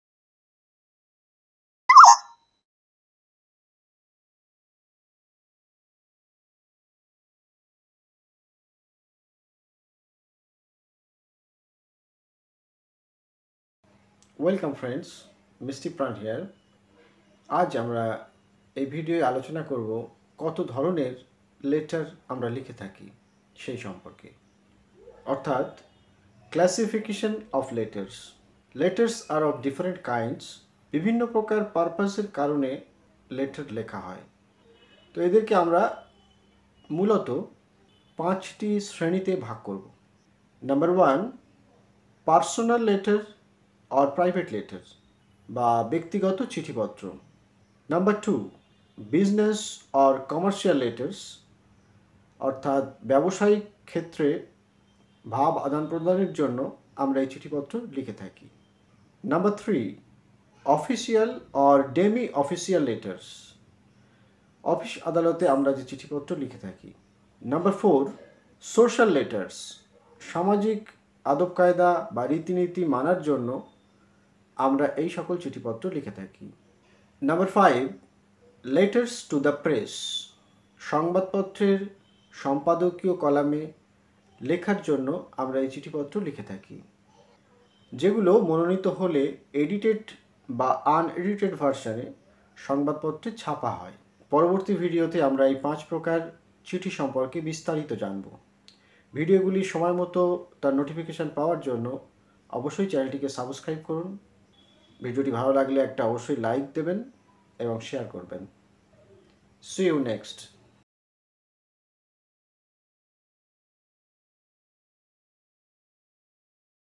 Letter writing in English || Types of LETTERS Writing || Writing Skills
Letter Writing - Letter Writing Types and Examples || A letter is a written message that can be handwritten or printed on paper. || Types of Letters ||There are various kinds of letters : These different types of letters are different in their subject, occasion and purpose. They have various criteria like format, length, vocabulary, language and other essentials
👋 Must Watch for Learners :-

 Types of Letters | How to write letter | Formal letter | Informal letter  More in the Video!

If you have any suggestions,questions or need any assistance/guidance then please write in the comment section below..